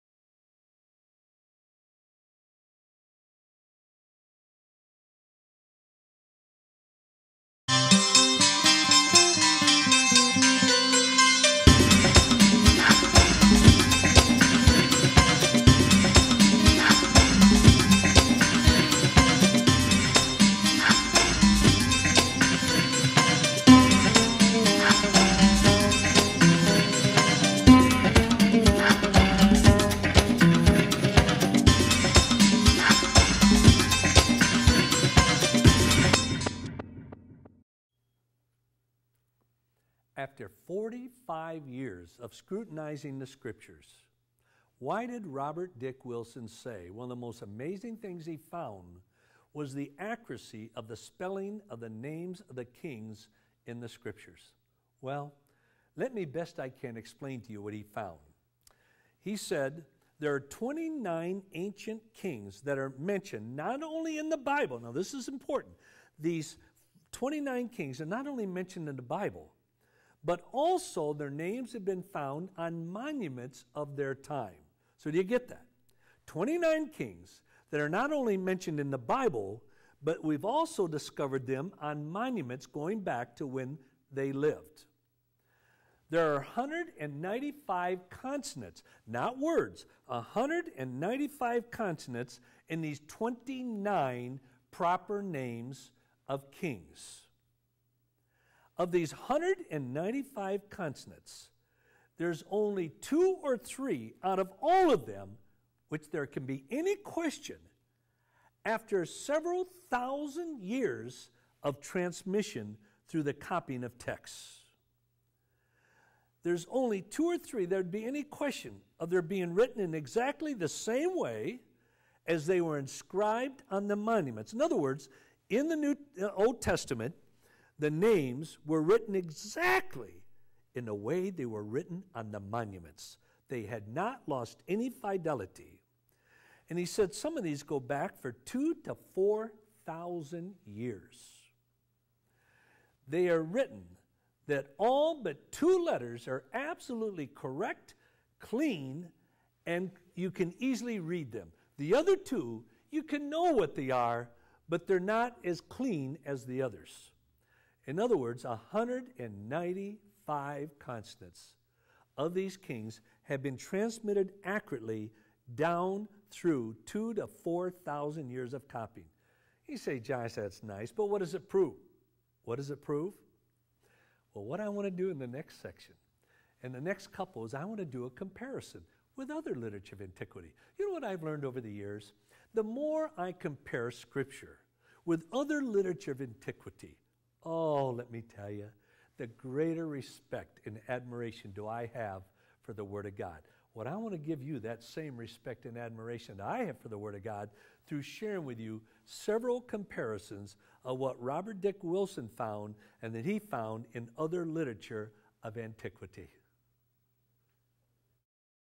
Old Testament: Robert Dick Wilson: Accuracy of Kings' Names | Is The Bible Reliable?
After 45 years of scrutinizing the Scriptures, Robert Dick Wilson said one of the most amazing things he found was the accuracy of spelling of the names of kings in the Scriptures. Why is this so significant? Listen and see!